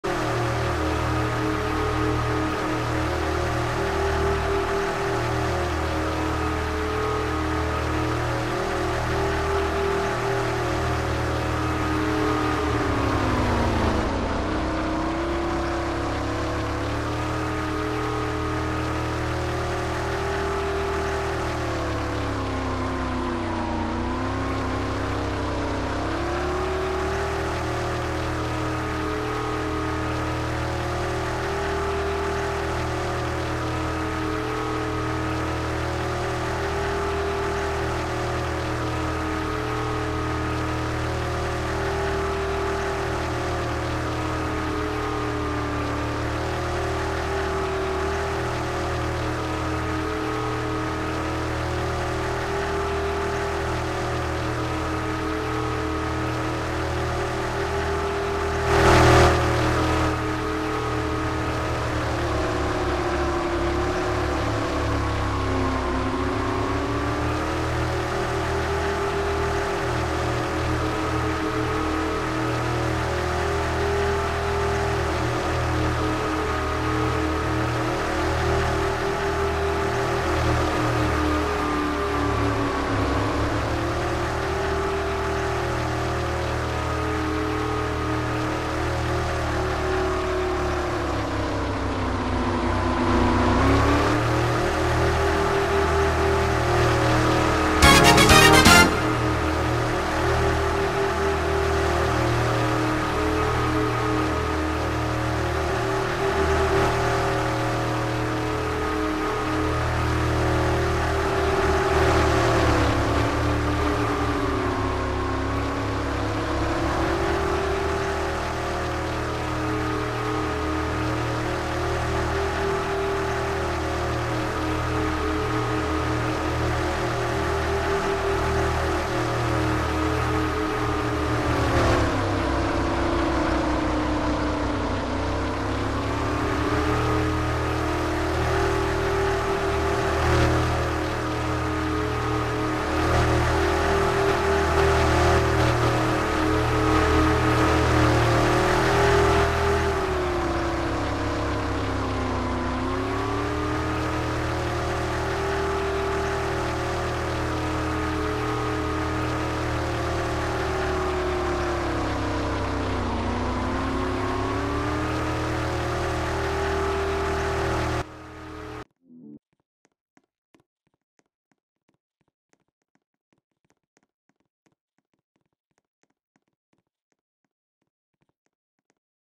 [ROSTOCK RACING} Uh-huh, "dirty" Ok Pal
Dunno why he decided to votekick me for being dirty, but at least I didn't get kicked

:kekw: